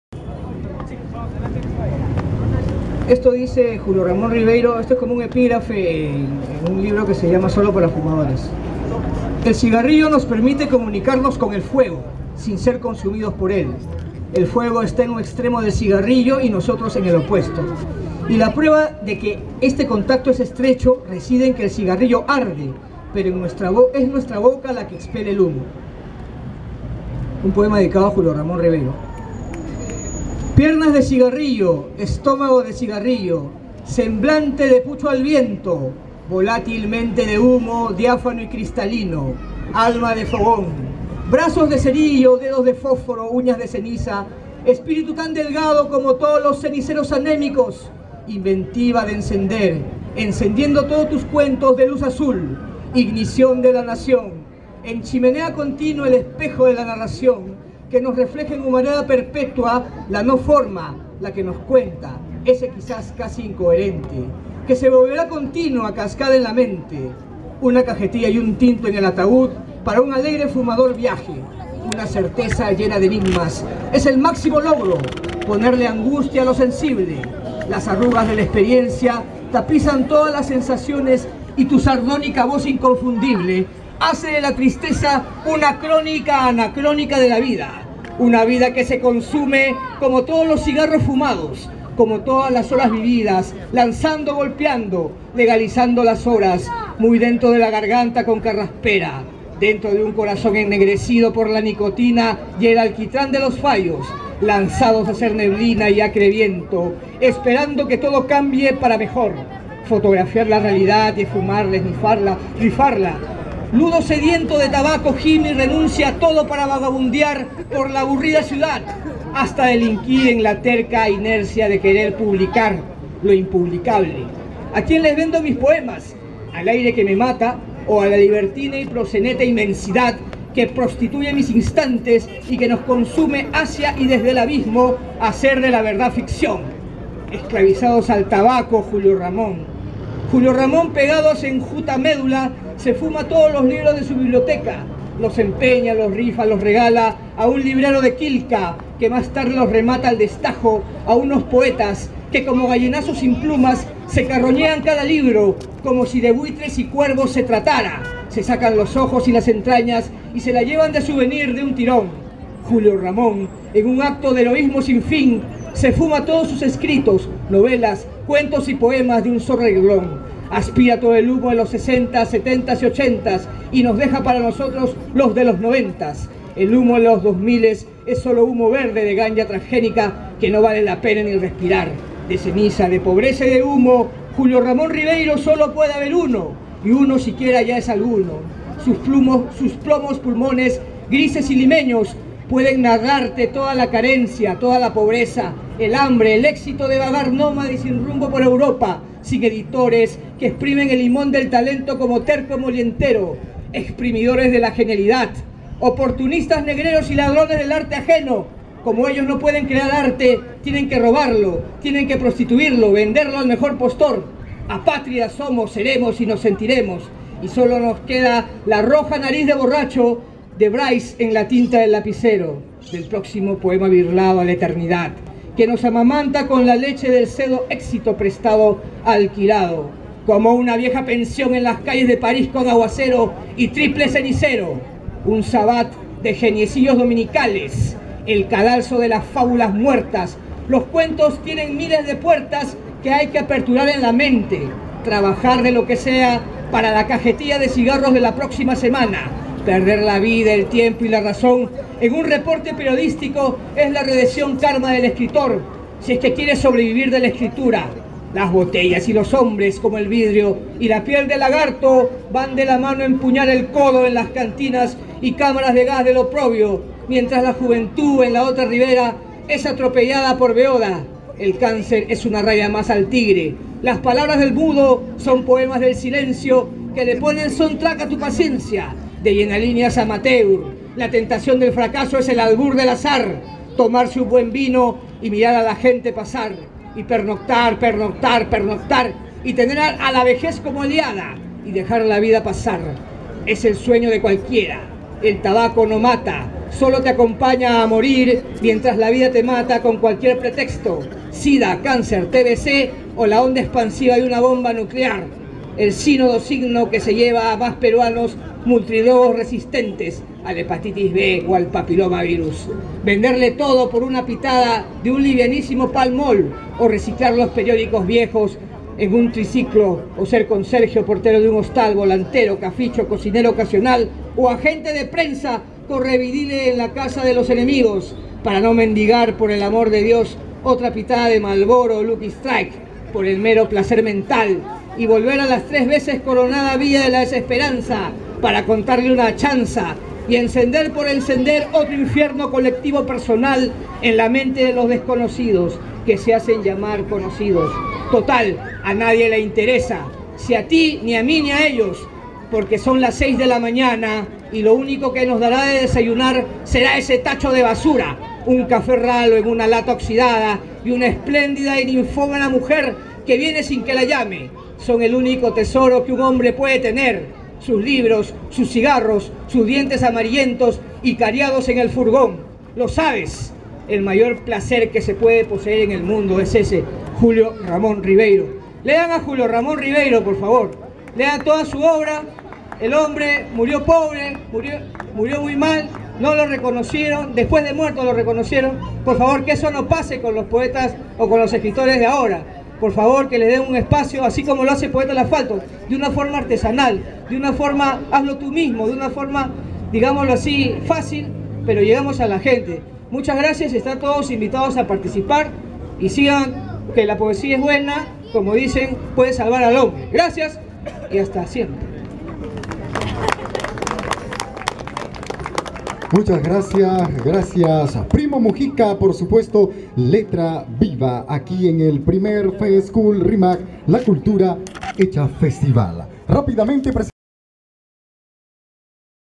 "PRIMO" MUJICA EN EL 1ER FEST KUL RIMAC (31-10-2015)
LEYENDO UN POEMA A  JULIO RAMÓN RIBEYRO EN EL 1er FEST KUL RIMAC --SÁBADO 31 DE OCTUBRE DEL 2015---